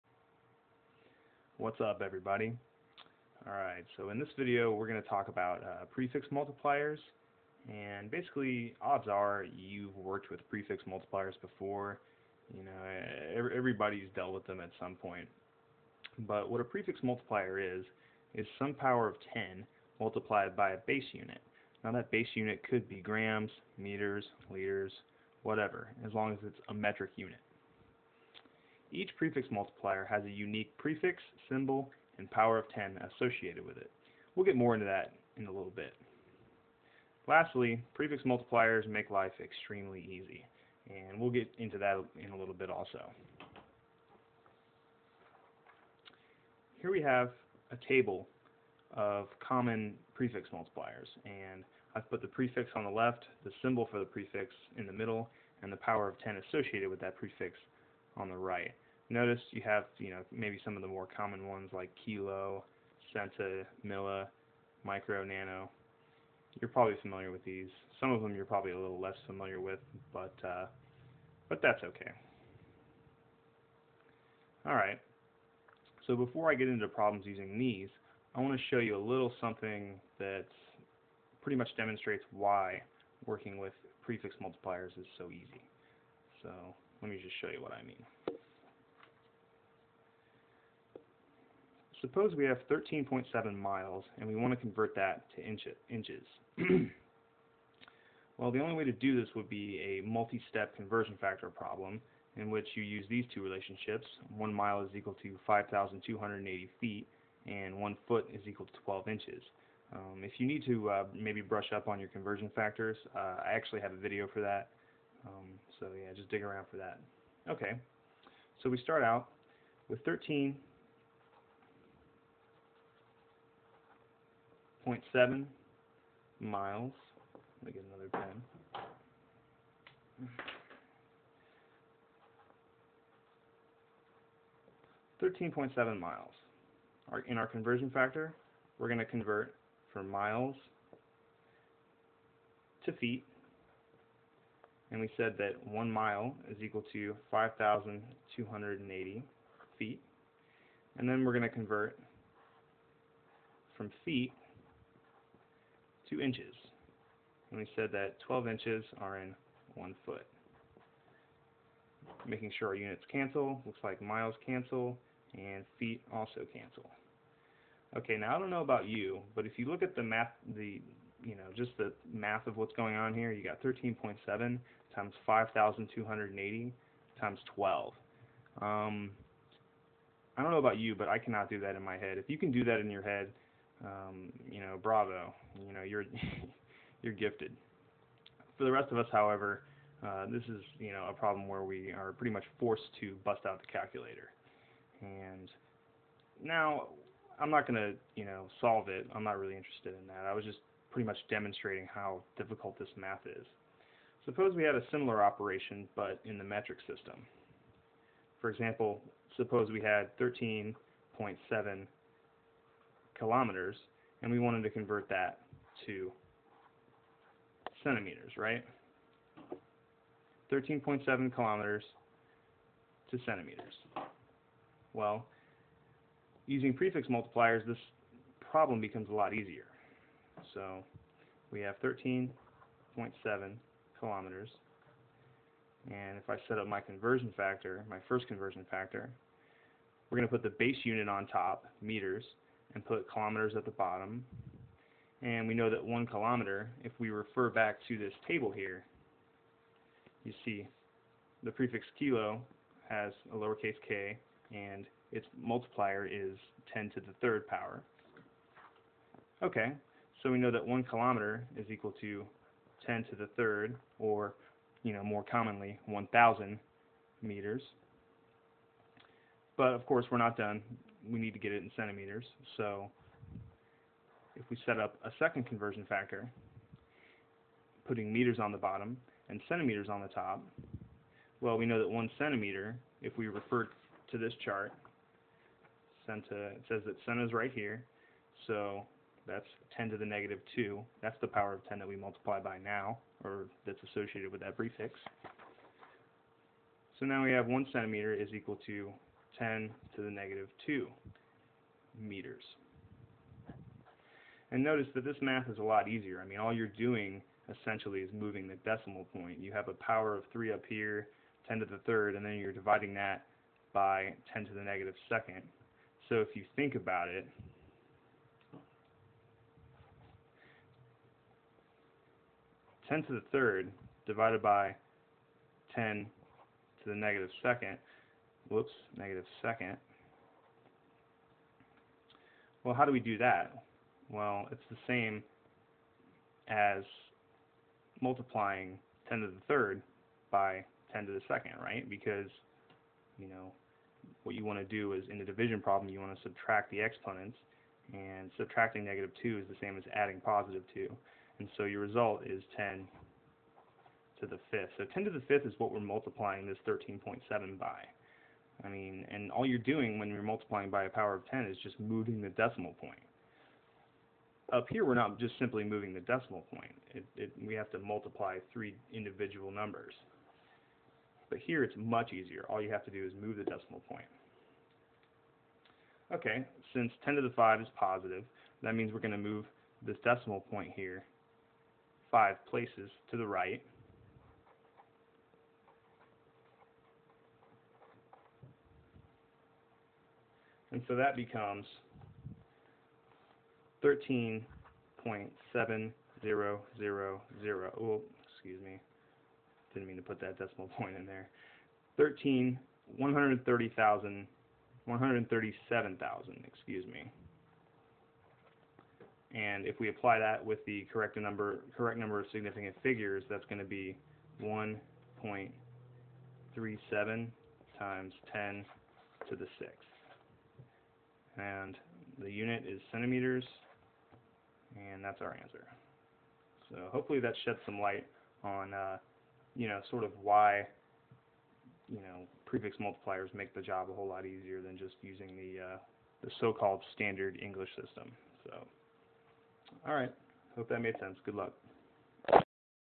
Prefix Multipliers
Basic Information on prefix multiplers.  How to convert between metric units of the same property (i.e. from kilometers to centimeters).

**In the example, the number 137,000 should be 1,370,000.  I've been trying to insert an annotation explaining this, but I'm having issues doing it.**